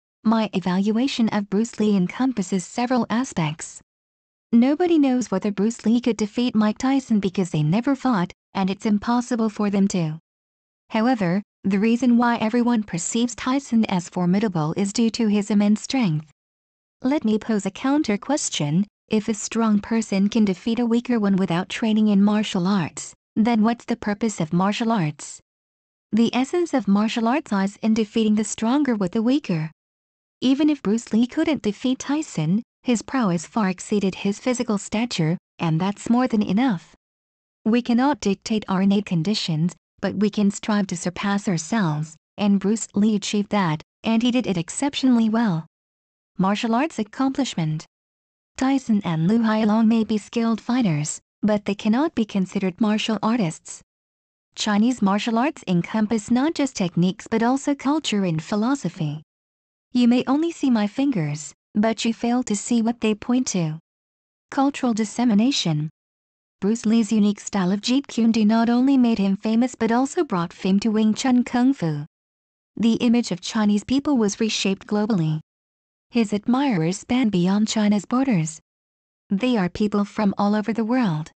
Bruce Lee VS Tyson: Beyond physical strength, the ultimate battle of martial arts souls!
This is what the Chinese Internet says about Bruce Lee.

Chinese people only know about Bruce Lee movies in the 1990s.